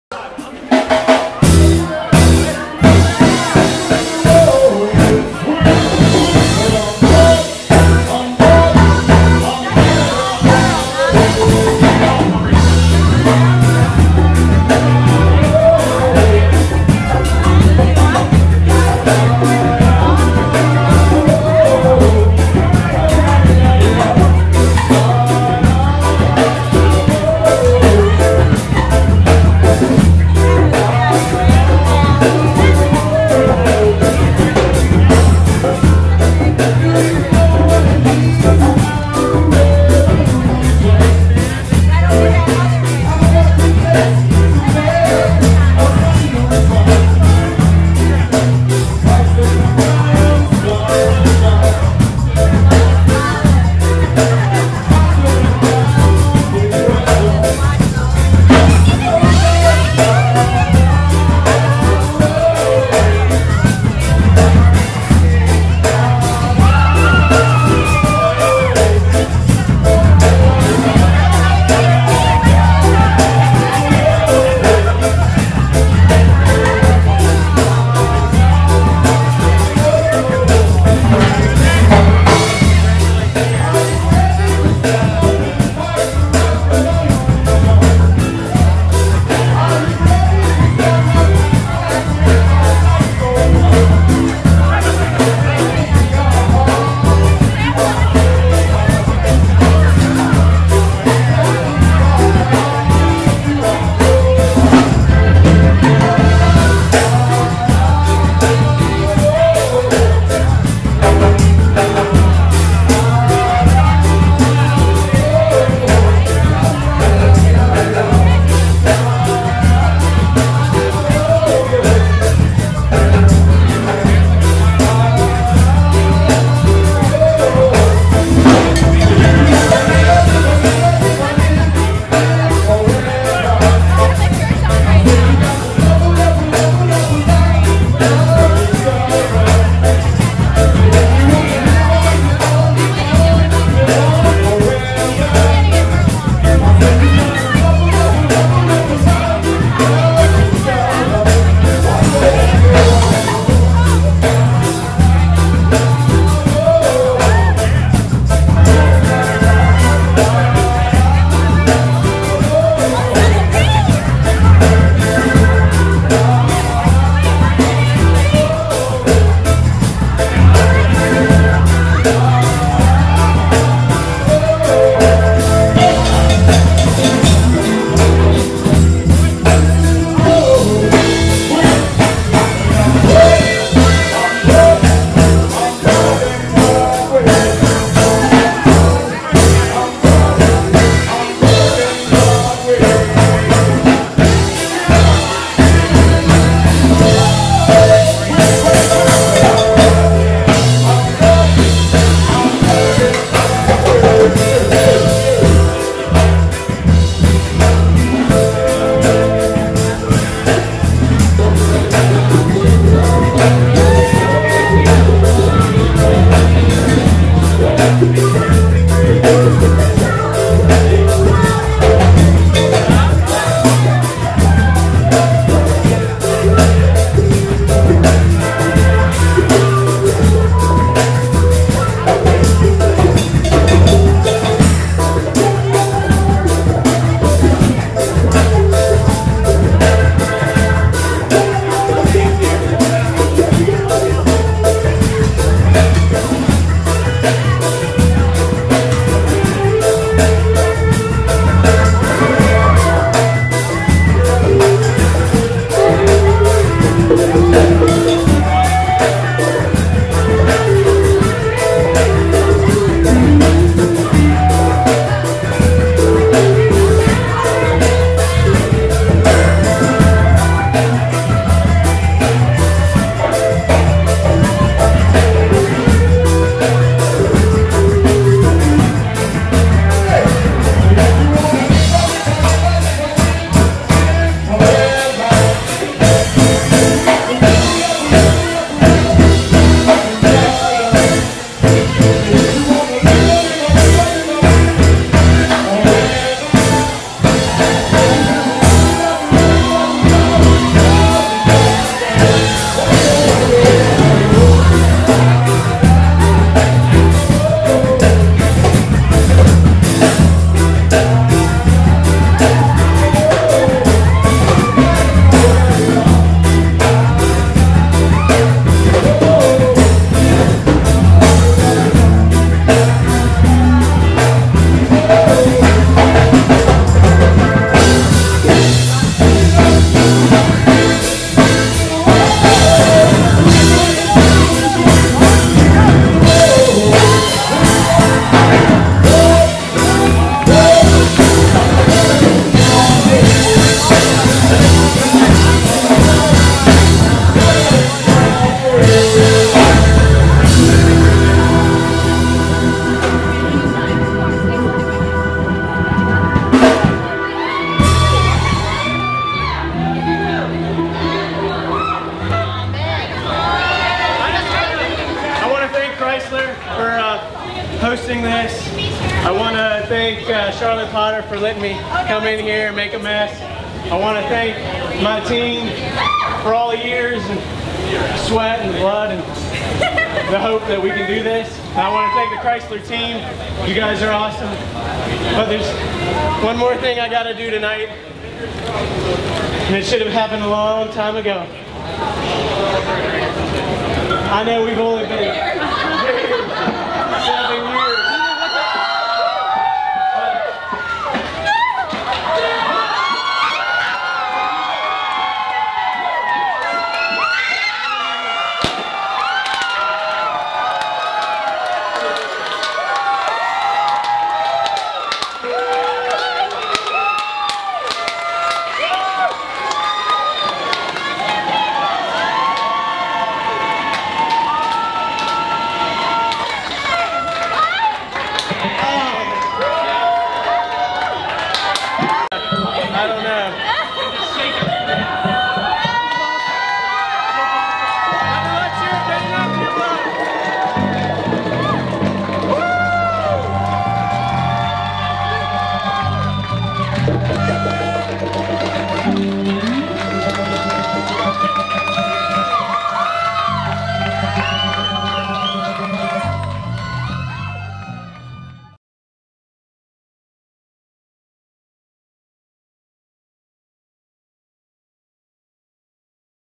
The Great Turtle Migration
Third Wednesday at the Chrysler Museum Glass Studio. Demonstration by Grant Garmezy. Music perfomance by Antero. Stay till the end for a special surprise.